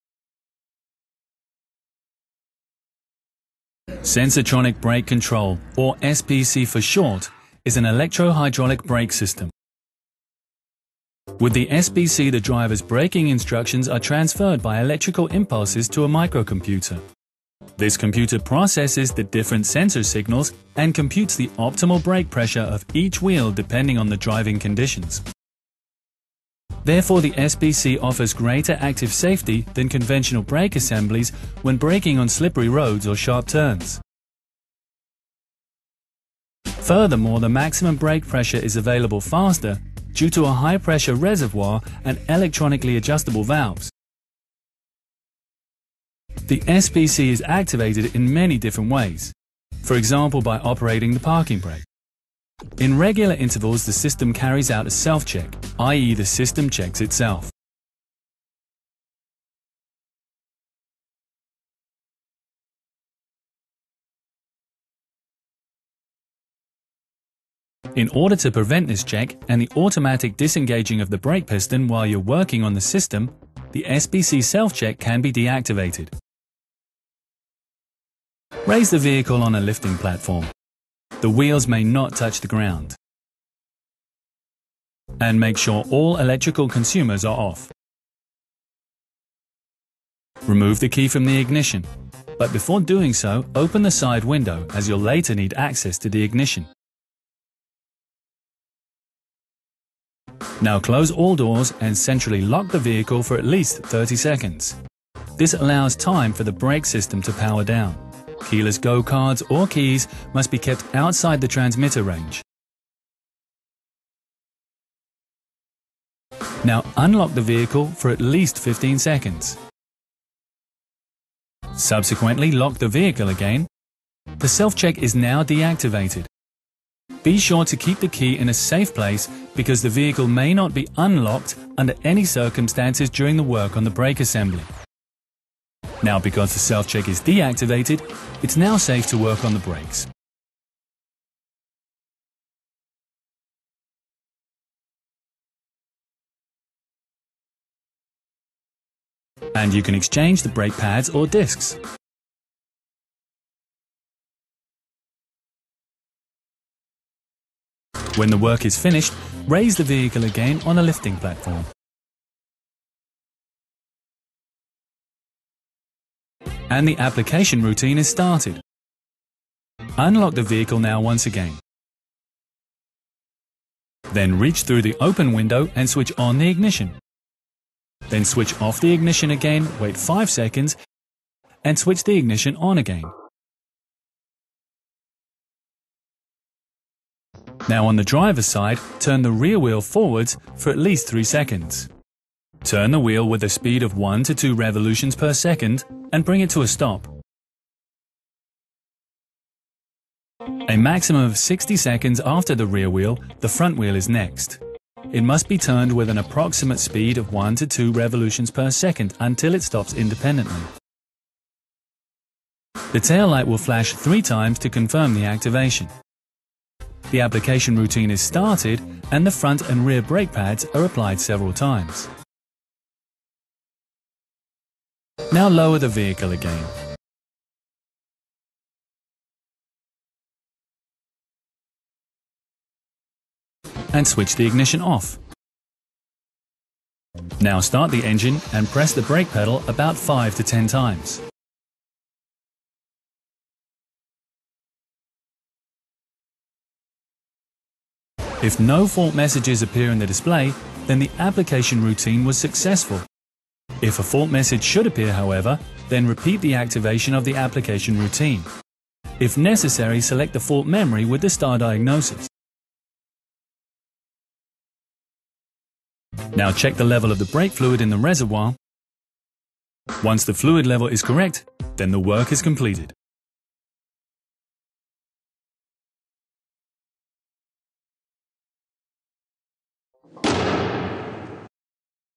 Mercedes-Benz SL - Prevent SBC self-test and apply brake pads | R230
Sensotronic Brake Control, also called SBC, is an electrohydraulic brake system.
With SBC, the braking commands of the driver are transferred to a microcomputer by electrical pulses.
This computer processes  the various sensor signals.
In addition, it calculates the optimal  braking pressure for each wheel  depending on the driving situation.
When braking in curves or on slippery road surfaces, SBC provides greater active safety than conventional brake systems.
The maximum braking pressure is available more quickly.
This is achieved with a high-pressure reservoir and electronically controllable valves.
SBC is activated by various wake-up events  e.g. when the parking brake is released.
The system then performs  a self-test at regular intervals.
This means that the system checks  itself for proper operation.
This SBC self-test must be deactivated when working on the system.
This prevents the test and automatic movement of the brake pistons.
Lift the vehicle with a vehicle lift.
The wheels may not touch the ground.
Switch off all electrical consumers.
Remove key from ignition lock.
Note: Leave the side window open.
This allows you to switch the ignition on and off later.
Globally lock the vehicle for at least 30 s.
This allows the brake system  run-on period to elapse.
Keyless Go cards and keys must be stored outside the transmission range.
Unlock the vehicle for at least 15 s.
Lock the vehicle again.
The self-test is then deactivated.
Store the key where it cannot  be accessed by others.
Note: The vehicle must not be unlocked under any circumstances when working on the brake system.
Since the self-test is deactivated, work can now be performed in the vicinity of the wheel brakes.
For example, brake pads or brake disks can be replaced.
Lift the vehicle again with a vehicle lift after the work is completed.
Activate the application routine  for the brake pads.
To do so, unlock the vehicle again.
Switch on the ignition through the open window.
Switch the ignition back off  and wait for 1 - 5 s.
Switch the ignition back on.
Turn the rear wheel on the driver side in the direction of travel for at least 3 s.
The wheel should turn at 1-2 rev/s.
Bring the wheel to a standstill.
Switch to the front wheel max. 60 s after finishing with the rear wheel.
Turn at 1 - 2 rev/s, until it brakes itself automatically.
The rear lamps flash three times to confirm successful activation.
The application routine is started and the front and rear brake pads are applied several times.
Lower vehicle again.
Switch off the ignition.
Start the engine.
Fully depress brake pedal 5 - 10 times.
If no fault message appears on the display, the application routine was successful.
If a fault message appears, repeat activation of the application routine.
Read out the fault memory with Star Diagnosis if necessary.
Finally, check the brake fluid level in the expansion reservoir.
If the fill level is OK, the work is complete.